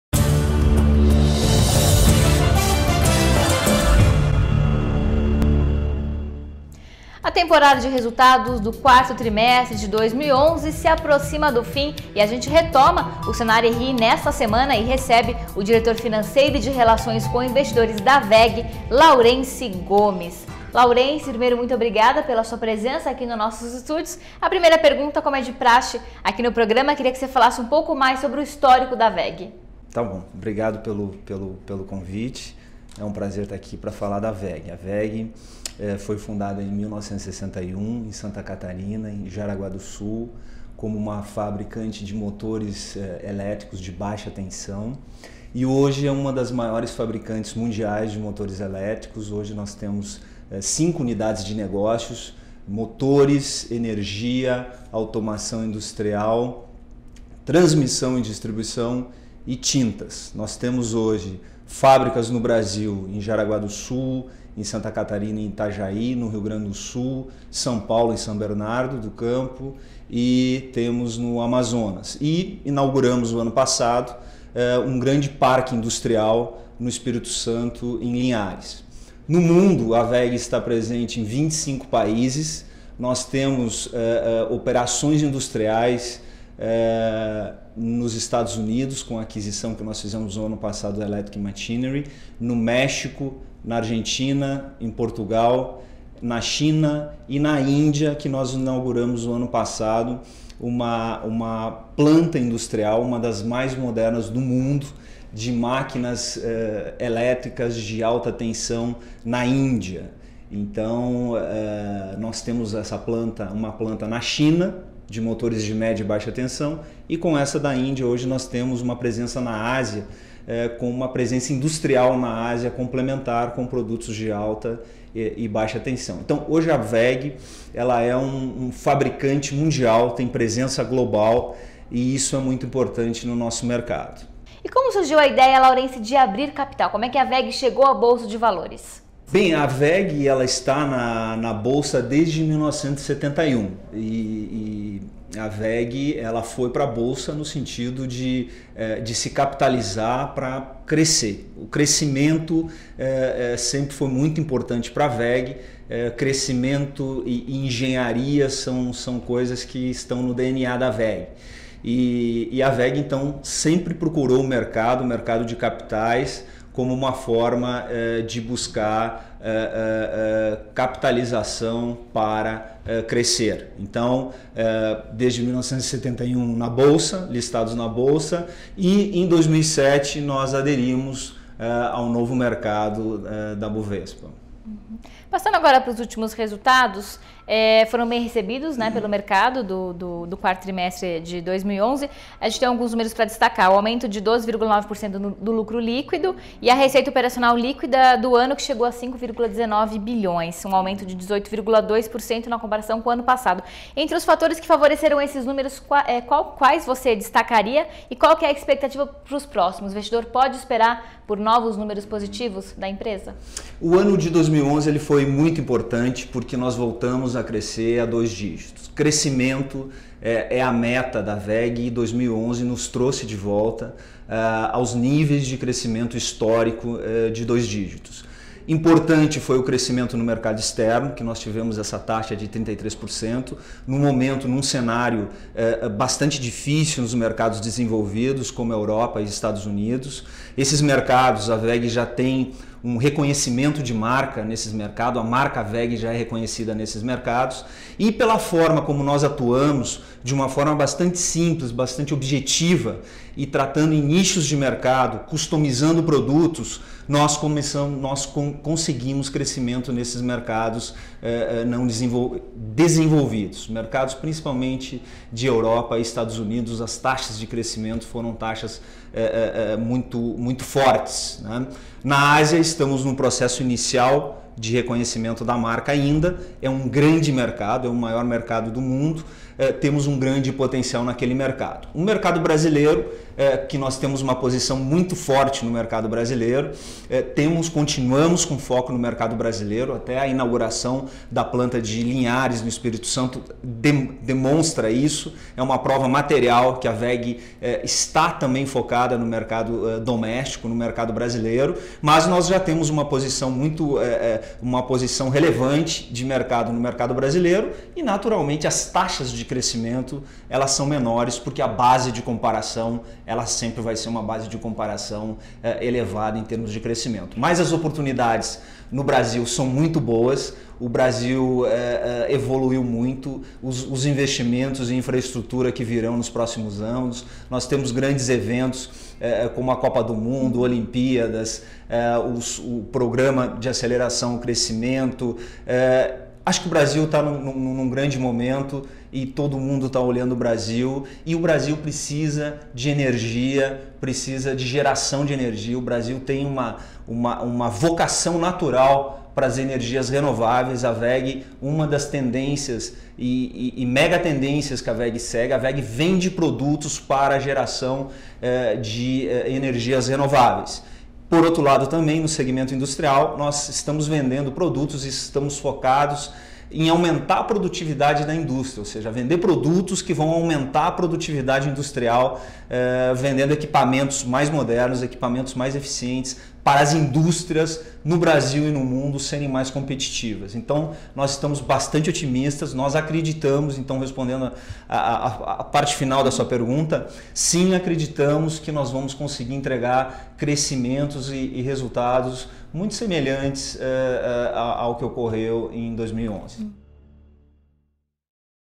Cenário RI: Investidor pode esperar por novos números positivos em 2012, diz Weg - PT 01
19 de março de 2012 • 16h20
Por: Heloisa Ferraz Finocchiaro

Após a temporada de resultados, e do período de silêncio das companhias listadas na bolsa, o Cenário RI está de volta. A edição desta semana recebe o Diretor Financeiro e de Relações com Investidores da Weg (WEGE3), Laurence Gomes.